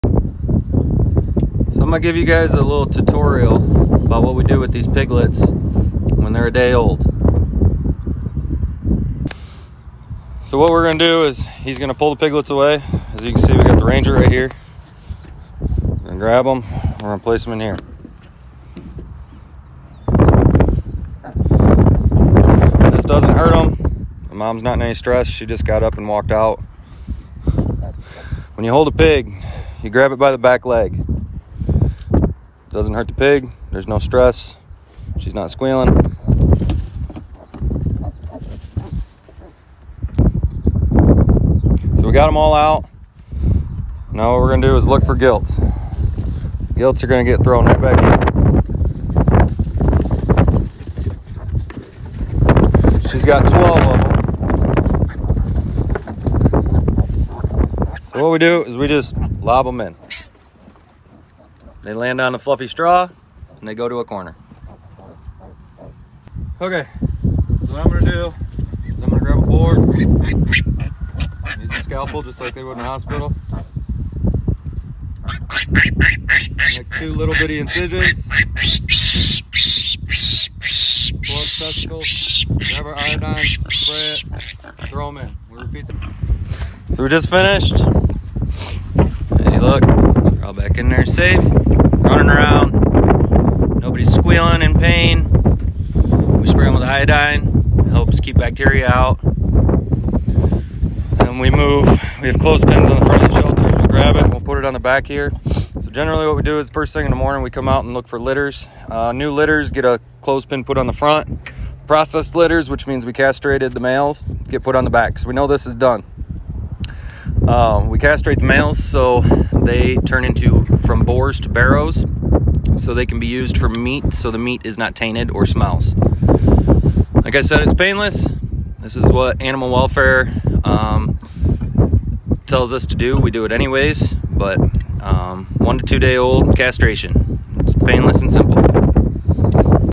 How and Why We Castrate Piglets
After baby animals are born on the farm, there are certain procedures we do as their caretakers to ensure they are healthy and monitor them while they are living on the farm, similarly to what parents or hospital workers do with their newborns when first born.  First, we count each animal and record any identifiable issues. We count the piglets again once they are weaned from their sow at 6 weeks old.  Counting the animals allows us to help identify any unseen issues throughout their lives due to predators or illnesses. After counting, we then separate the males and females briefly, returning the females to their shelter and keeping the males for castration. We castrate the males so they can be raised up for meat.  If we did not, the meat would smell and taste poorly.  Take a look at our step-by-step process castrating the males, it's a fairly simple procedure that doesn't keep the animals in much pain.  After some experience, this process becomes an easy step to accomplish as a farmer.